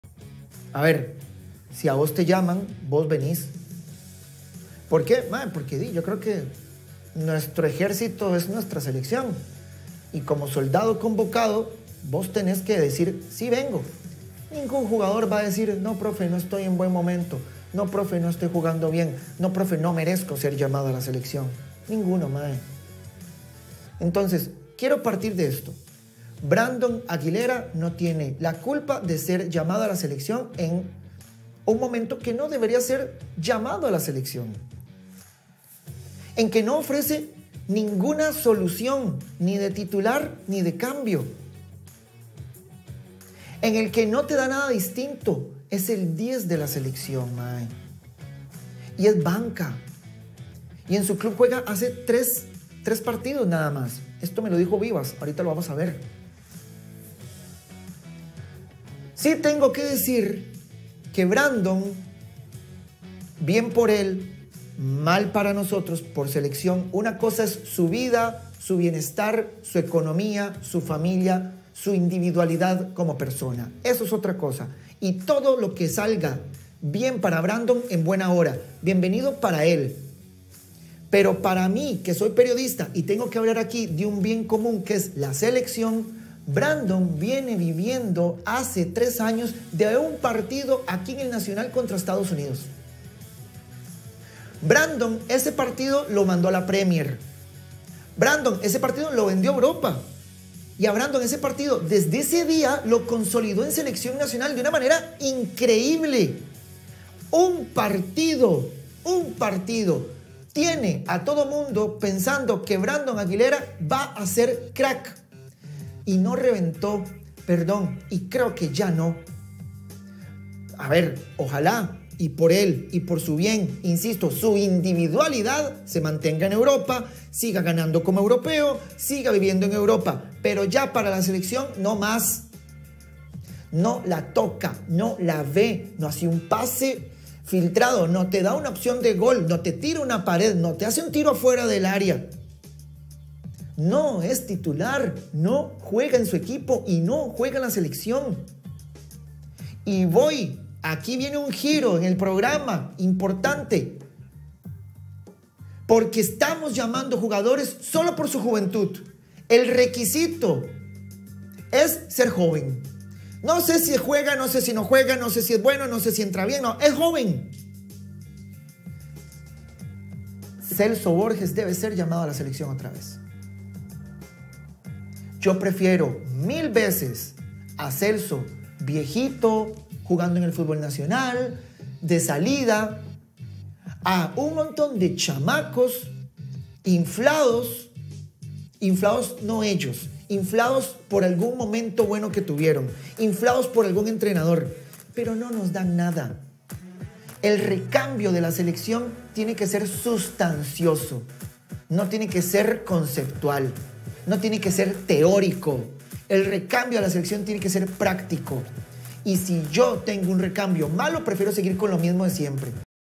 Brandon Aguilera se quedó en promesa
Josué Quesada cree que Brandon Aguilera no es el 10 que necesita la selección